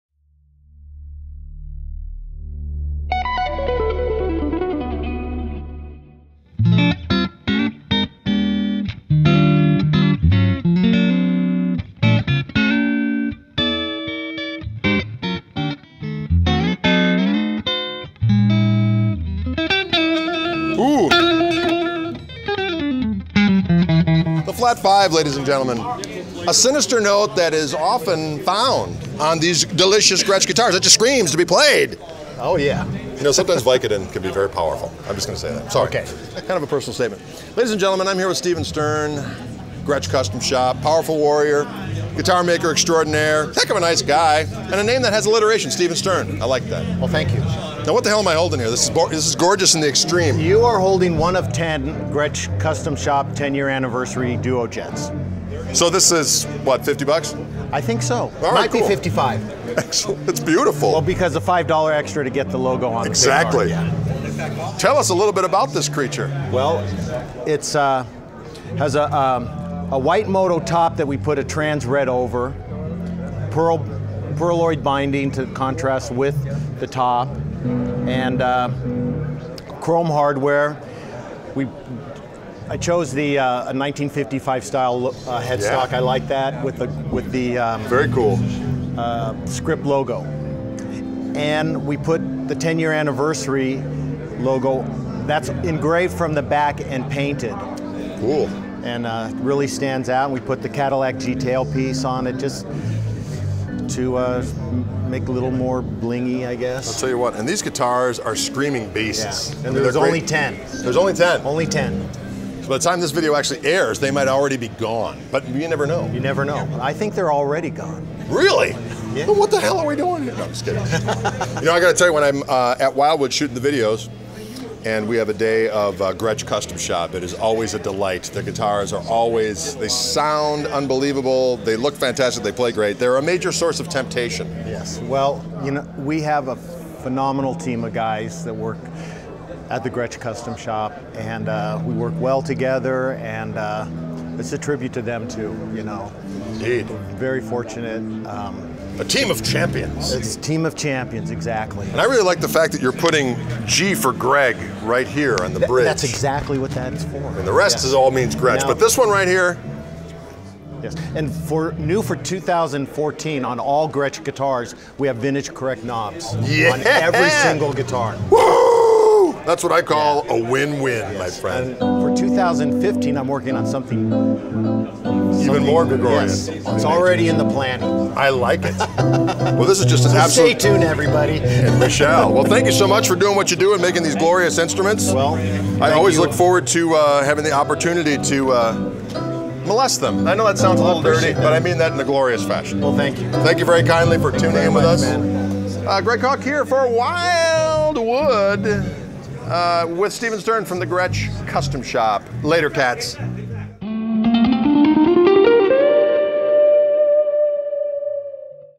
Gretsch Masterbuilder Stephen Stern • NAMM 2014
Here we are again, ladies and gentlemen!  The 2014 Winter NAMM Show is upon us, and we're pulling out all the stops!  In this video we sat down with legendary Gretsch Guitars Masterbuilder Stephen Stern to talk about what's new in the world of Gretsch Guitars, what cool new creations he's been working on, and to talk about and play the stunning Gretsch Custom Shop 10th Anniversary Duo Jet!  Wham bam, check it out!